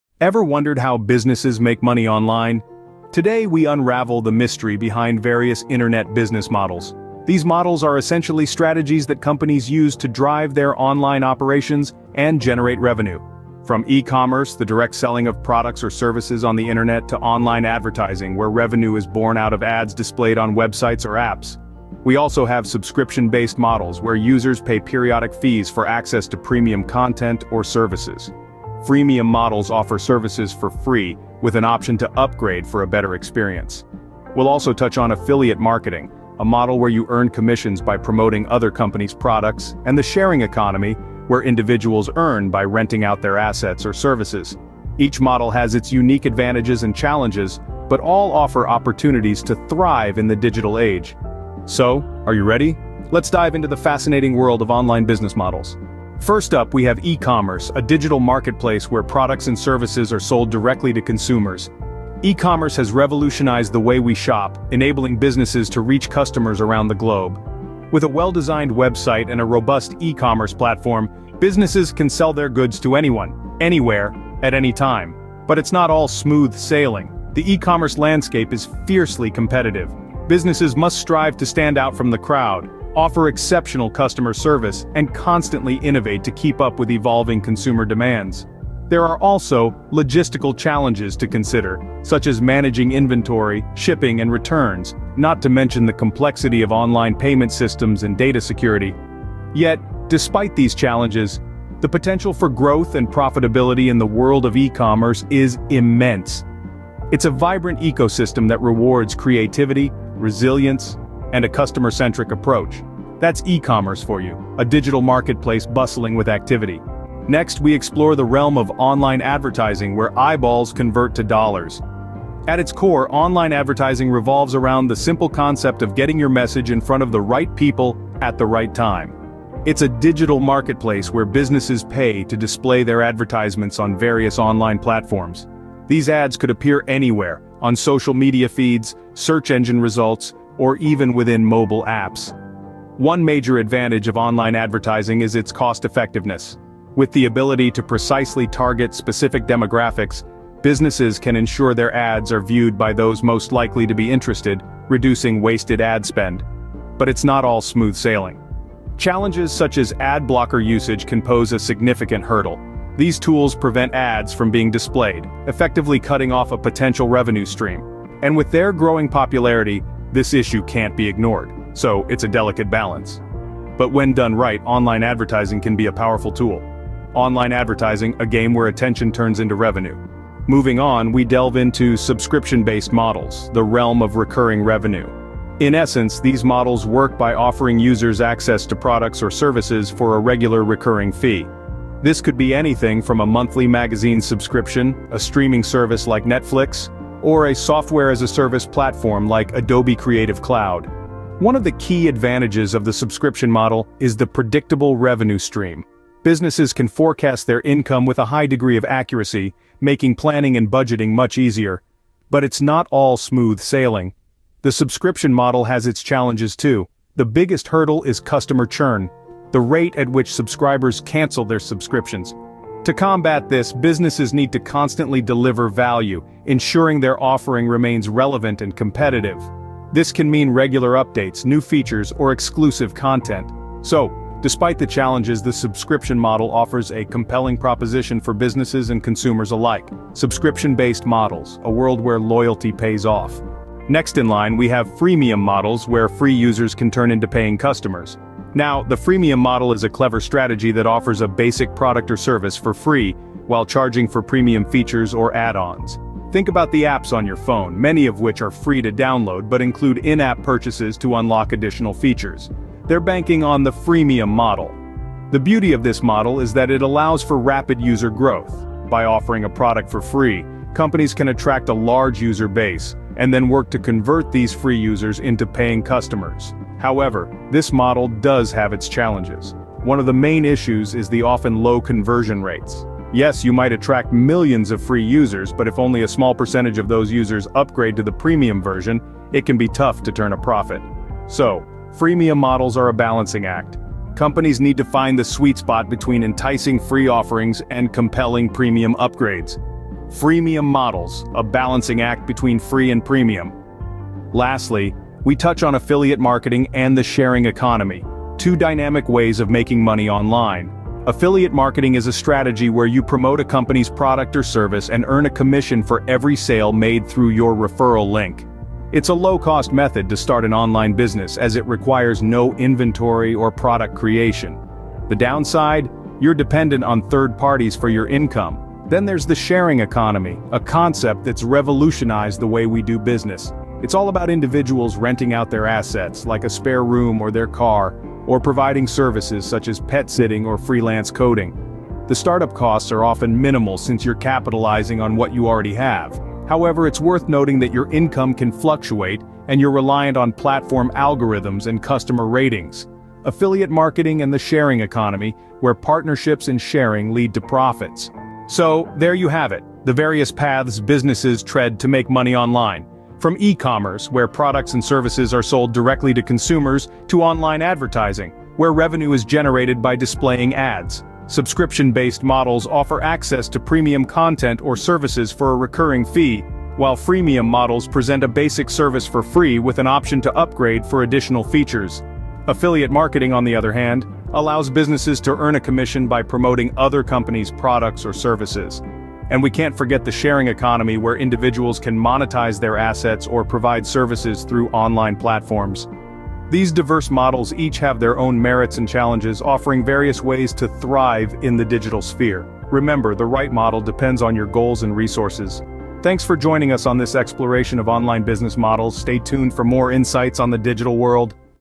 9 INTERNET BUSINESS MODELS MAKING MILLIONS | 2024
9 INTERNET BUSINESS MODELS MAKING MILLIONS | 2024

Discover the top 9 internet business models that are making millions in 2024. Learn about the key strategies and techniques used by successful online businesses to generate substantial revenue. Whether you're a beginner looking to start your own online venture or an experienced entrepreneur seeking new ideas, this video will provide valuable insights into the world of internet business. Stay ahead of the competition and unlock the secrets to financial success with these proven business models.